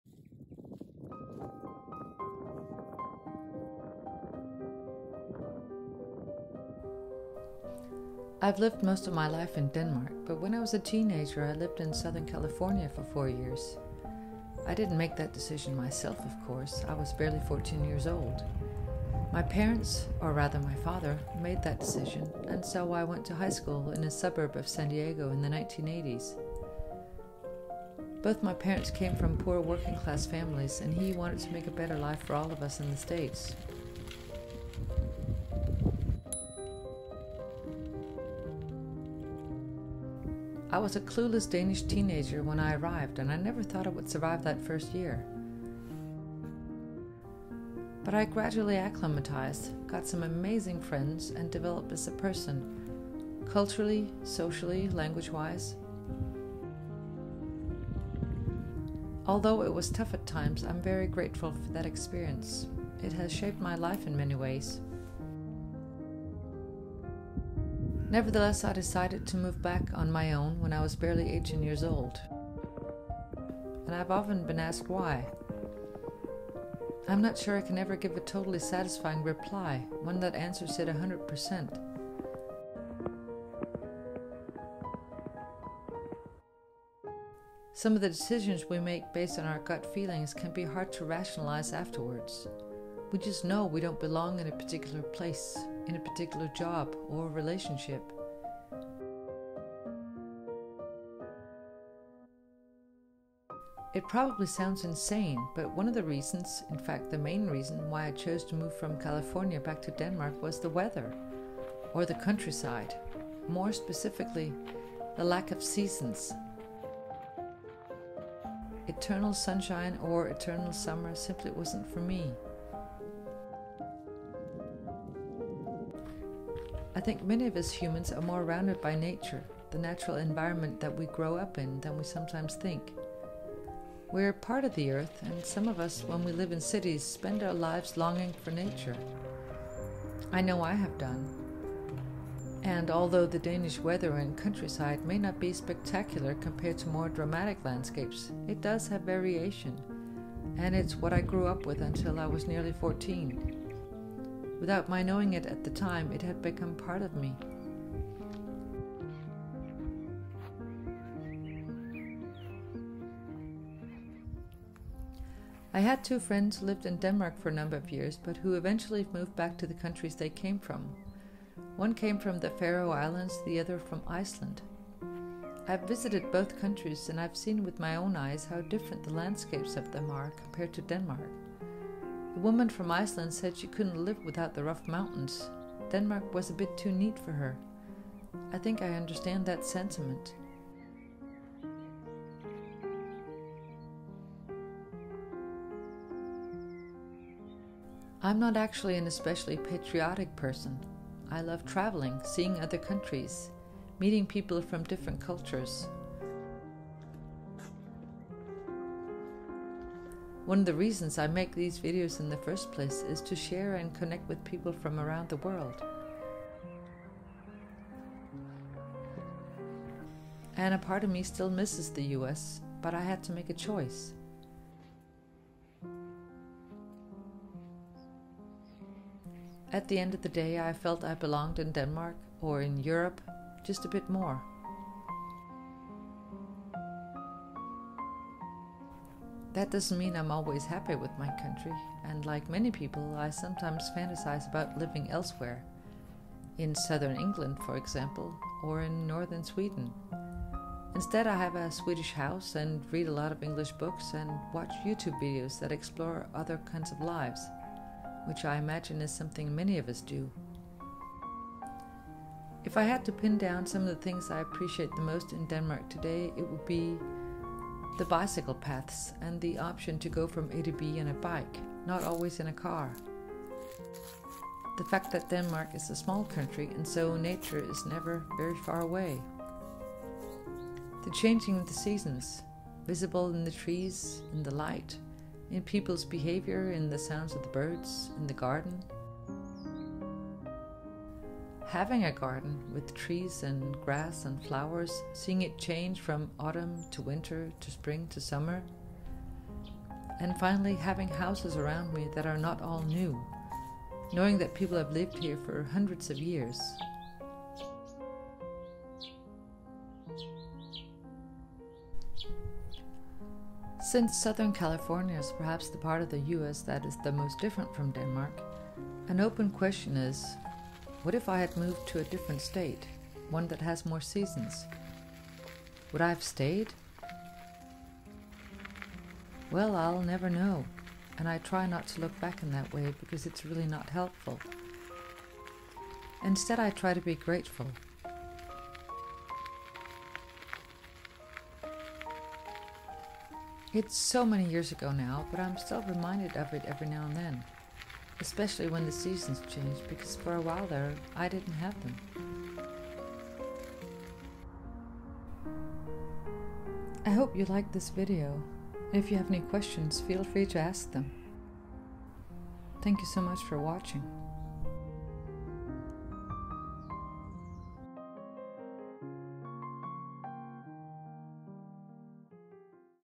Why I moved to the US and back to Denmark 4 years later
At one point in my life, I lived in the United States. Then I moved back to Denmark. Why? It's a question I've asked myself - and been asked - many times. In this video, I'll try to answer it, partly at least, with some images from Denmark. I wonder if some of you have similar experiences?
Always grateful for having you here. Much love!